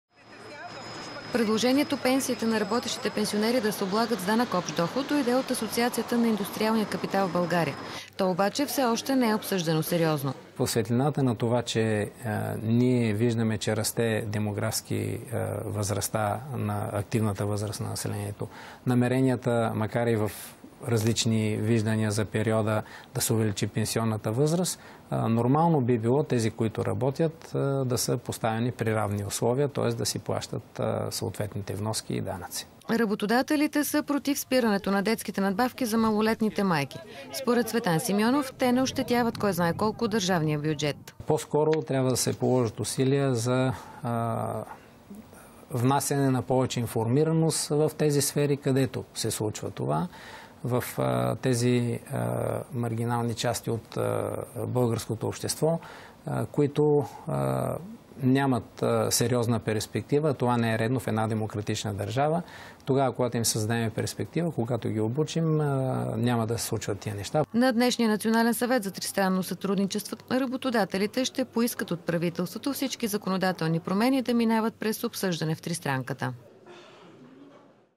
23 02 2015 TV+ Цветан Симеонов: всичики законодателни промени да минават през НСТС
Коментар на председателя на БТПП Цветан Симеонов, непосредствено преди нзаседанието на НСТС

Всичики законодателни промени да минават през НСТС настояват работодателските организации